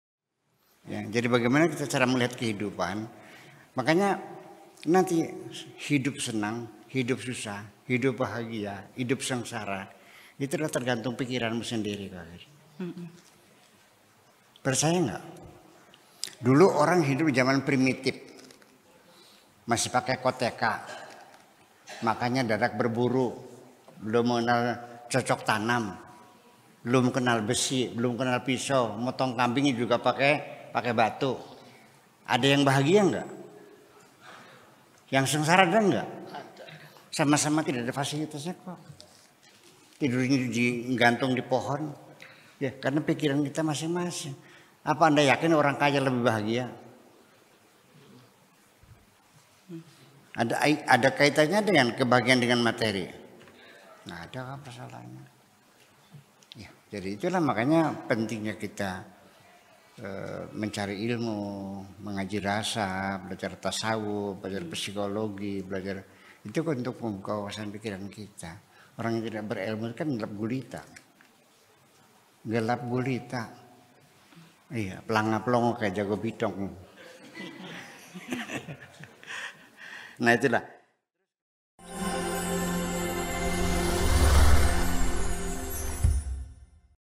Rahasia kebahagiaan karena pikiran
Kunci kebahagiaan seseorang adalah berasal dari dirinya sendiri yaitu bagaimana semampu dia mengolah pikirannya agar dia mendapat kebahagiaan.